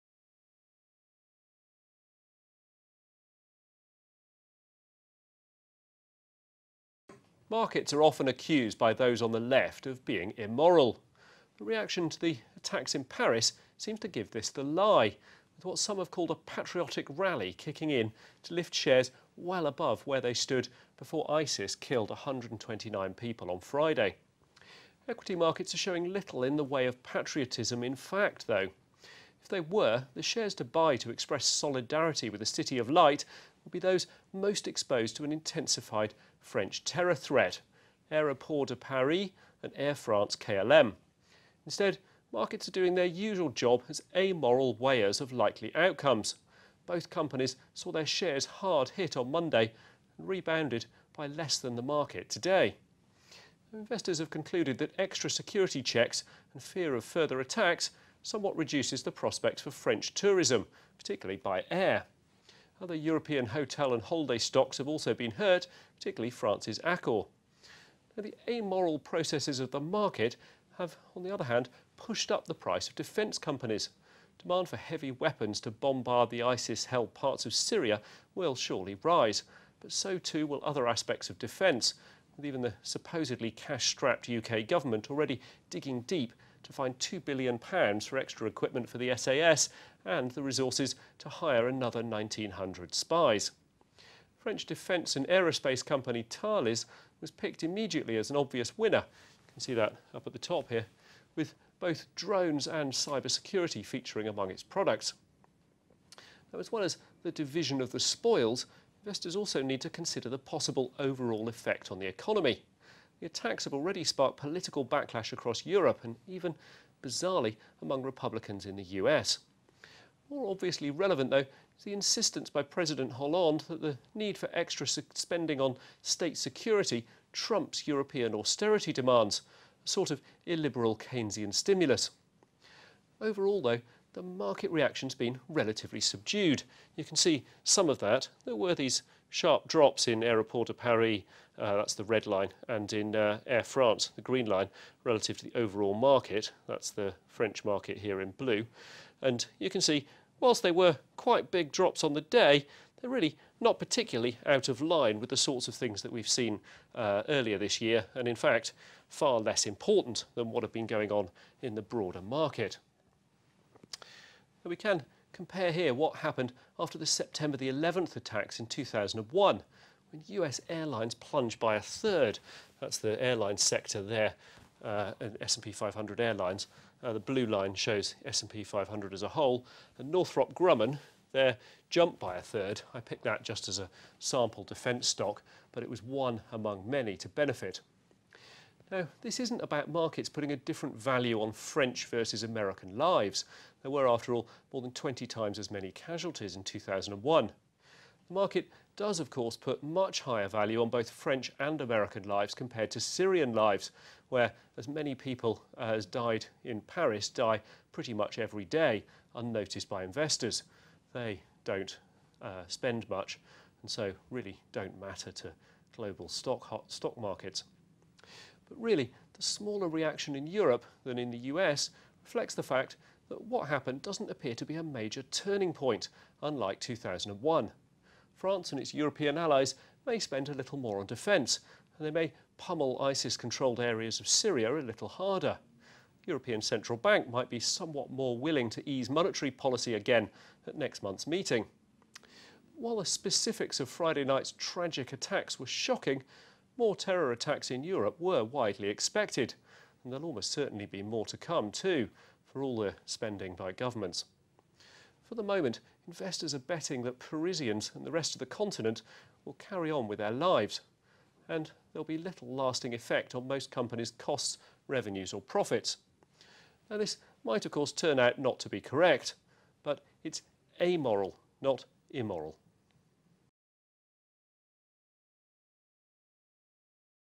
Amoral markets I Short View
Shares have risen in what some are calling a “patriotic rally” after the Paris attacks. The FT’s James Mackintosh says the market is performing its usual amoral weighing up of winners and losers, and unlike Paris, shares have not been terrorised